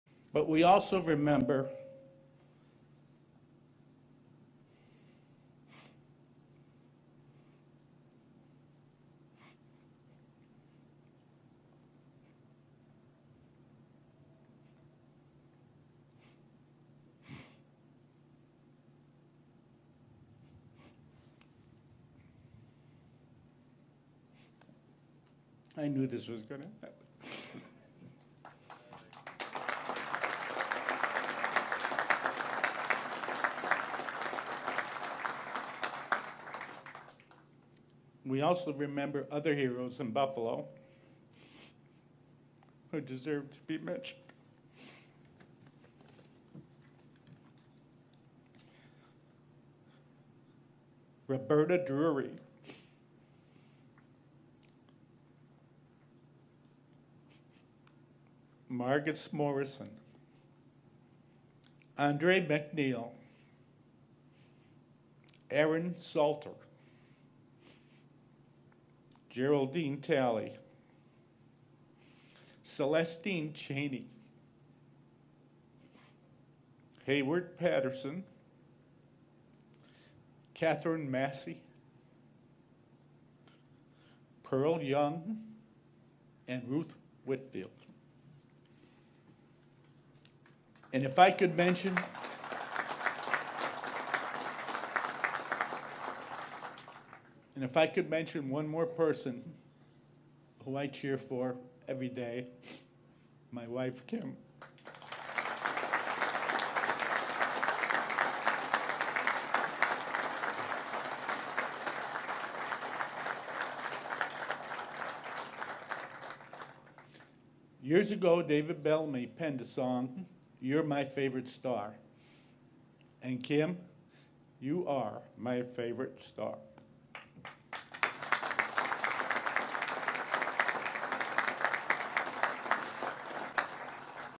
Bills owner Terry Pegula gets emotional at stadium groundbreaking while honoring 5/14 victims and wi
Bills owner Terry Pegula gets emotional at stadium groundbreaking while honoring 5/14 victims and wife, Kim